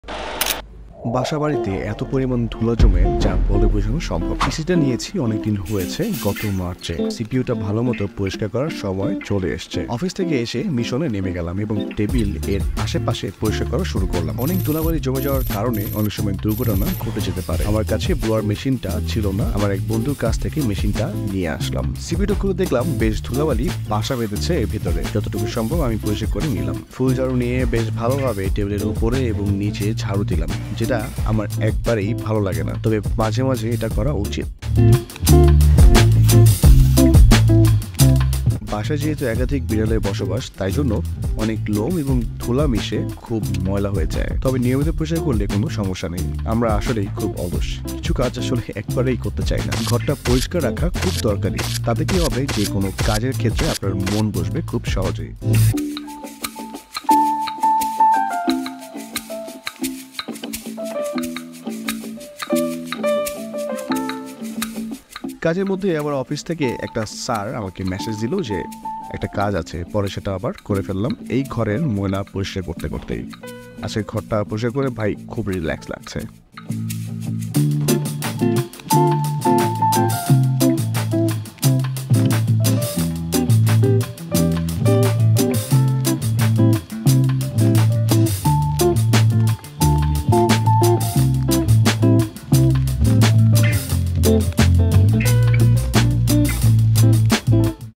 𝗦𝗮𝘆 𝗚𝗼𝗼𝗱𝗯𝘆𝗲 𝘁𝗼 𝗖𝗼𝗺𝗽𝘂𝘁𝗲𝗿 𝗦𝗹𝗼𝘄𝗱𝗼𝘄𝗻: 𝗔 𝗣𝗖 𝗖𝗹𝗲𝗮𝗻𝗶𝗻𝗴 𝗗𝗔𝗬
Is your computer running slower than a snail? Are you tired of waiting for it to load simple tasks? In this video, I'll show you how to give your computer a deep cleaning and boost its performance. We'll cover everything from removing junk files and viruses to optimizing your settings. Say goodbye to computer slowdown and hello to a faster, smoother experience!

I am very lazy at doing this work. I had to do it without wasting the time anymore. Whatever you do try to clean your PC once in a six month. Where I live, here is too much dust and I have four cats. Perhaps I have to live with the circumstances.